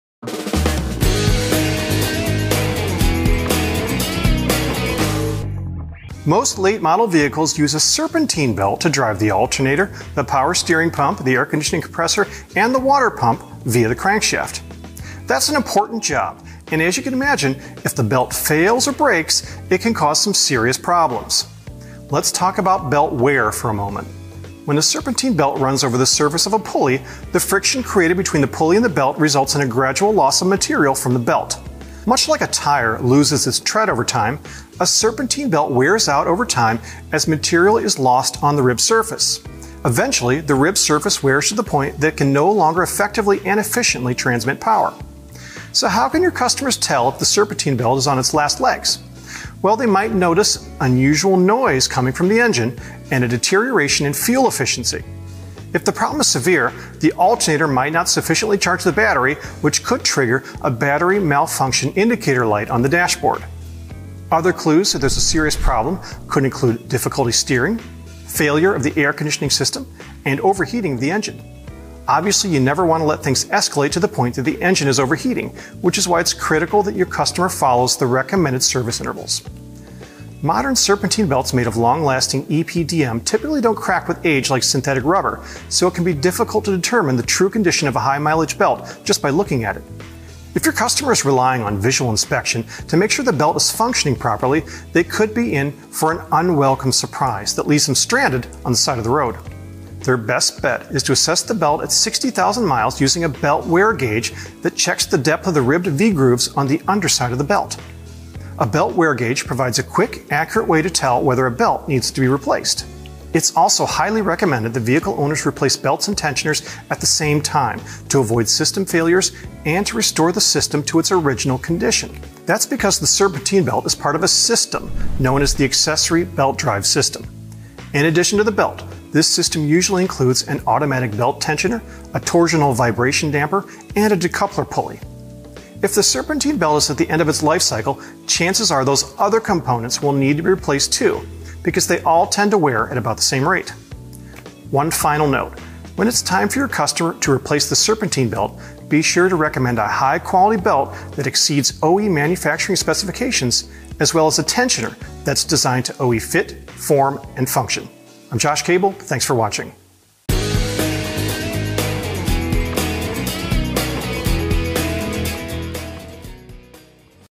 Is It Time to Replace the Serpentine Belt?
How can you tell if a serpentine belt is on its last legs? Here are a few tips.